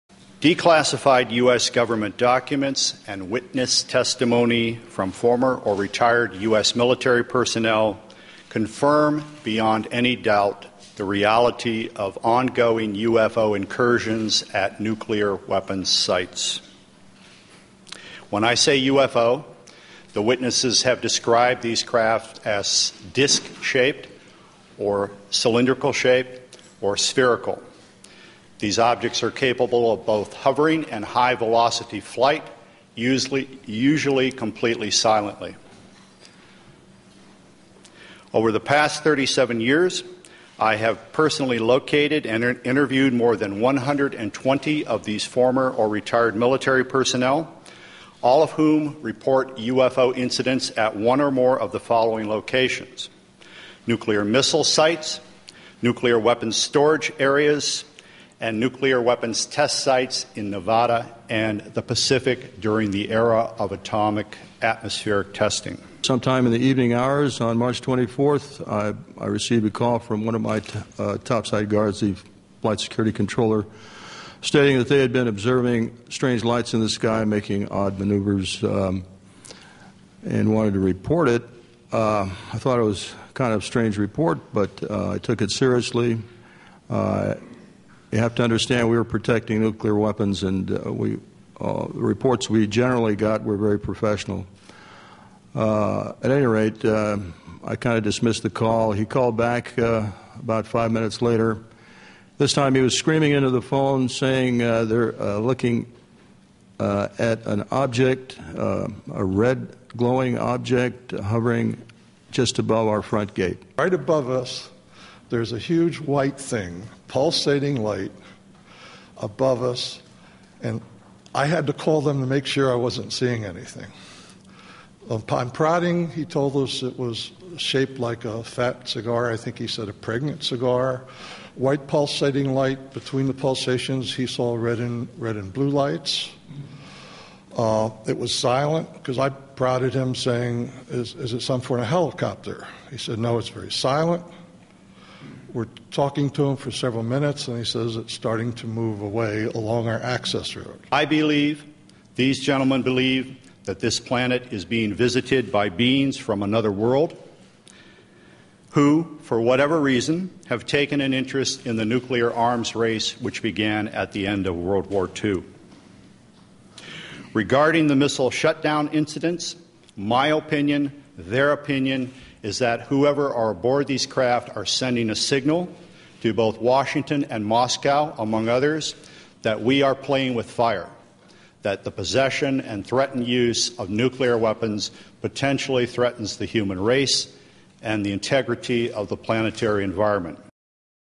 Epoch Opera : ufos&nukes
Seems like these two themes are regarded as "top secret" or "highly sensative" or "matter of international security"